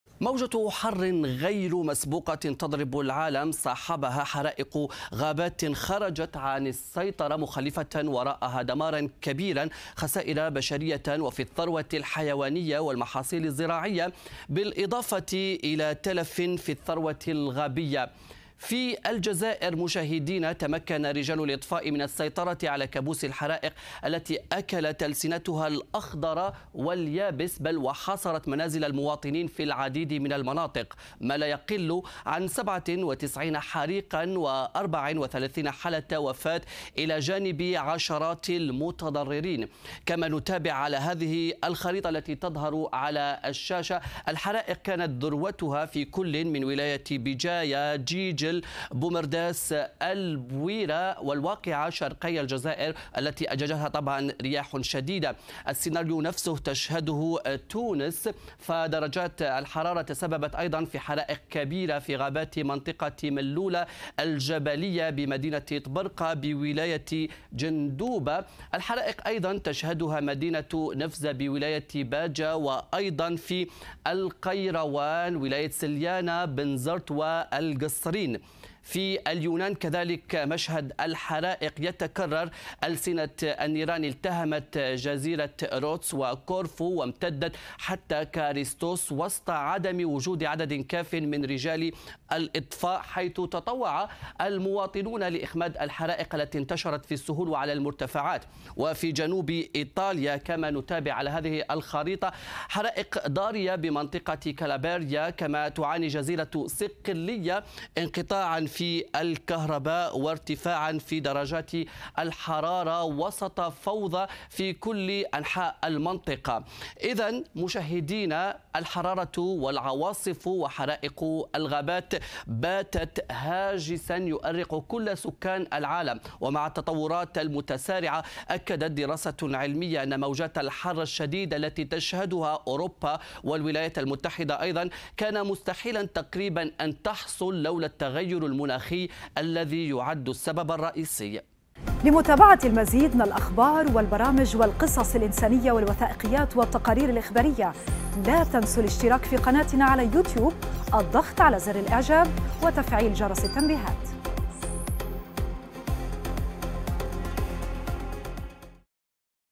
حرائق الغابات تخرج عن السيطرة في عدة دول
حرائق الغابات تخرج عن السيطرة في عدة دول.. و الجزائر تسجل 34 وفاة وعشرات المتضررين
لمعرفة أحدث المستجدات.. عبر البث المباشر من هنا ⤵
وبإمكانكم متابعة العربية على منصات التواصل